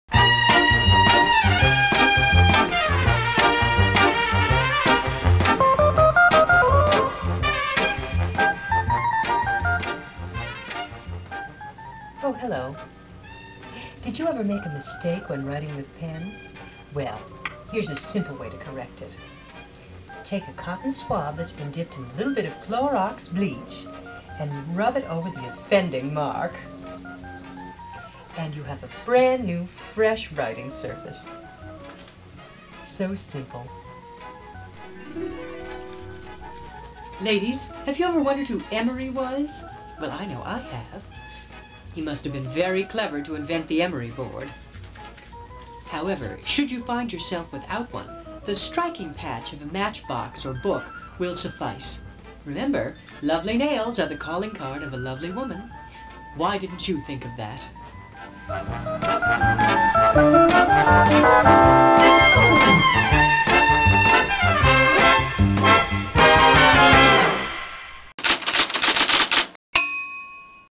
Tips for Letter Writing and Manicures
Brini shares some tips for erasing ink and keeping your nails lovely if you don't have an emery board handy.